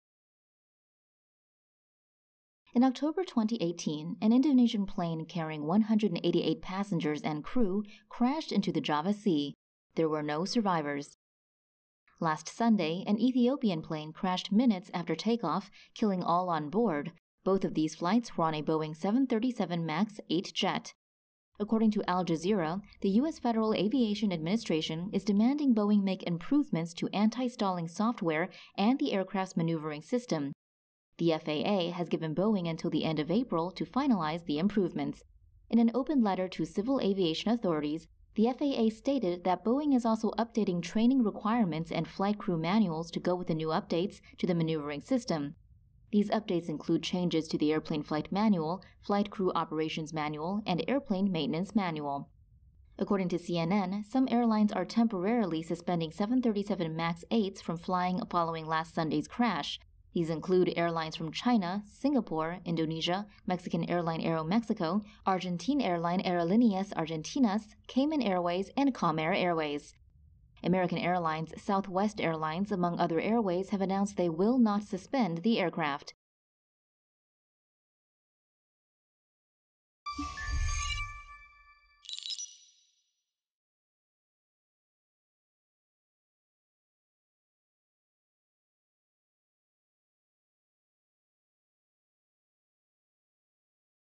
FAA demands Boeing improve 737 MAX 8 model
US regulators are demanding Boeing make modifications to improve the 737 MAX 8 following two crashes of the same model.

According to Al Jazeera, the US Federal Aviation Administration is demanding Boeing make improvements to anti-stalling software and the aircrafts' maneuvering system. The FAA has given Boeing until the end of April to finalize the improvements.

In an open letter to civil aviation authorities, the FAA stated that Boeing is also updating training requirements and flight crew manuals to go with the new updates to the maneuvering system.

These updates include changes to the airplane flight manual, flight crew operations manual and airplane maintenance manual.

According to CNN, some airlines are temporarily suspending 737 MAX 8s from flying following last Sunday's crash.

These include airlines from China, Singapore, Indonesia, Mexican airline Aeromexico, Argentine airline Aerolíneas Argentinas, Cayman Airways and Comair Airways.

American Airlines, Southwest Airlines among other airways have announced they will not suspend the aircraft.

According to the FAA, there are currently 387 737 MAX 8 and 9 jets flying worldwide, 74 of which are registered in the U.S.

RUNDOWN SHOWS:
1. Lionair plane crash
2. Ethiopian Airlines crash
3. US Federal Aviation Administration demands Boeing make changes
4. Boeing updating training requirements and flight crew manuals
5. Other manuals that will be updated according to Boeing
6. Airlines and countries that have suspended  737 MAX 8

VOICEOVER (in English):
"In October 2018 an Indonesian plane carrying 188 passengers and crew crashed into the Java Sea. There were no survivors."

"Last Sunday, an Ethiopian plane crashed minutes after take-off killing all onboard. Both of these flights were on a Boeing 737 MAX 8 jet."

SOURCES:
Al Jazeera, Quartz, FAA,  Boeing, CNN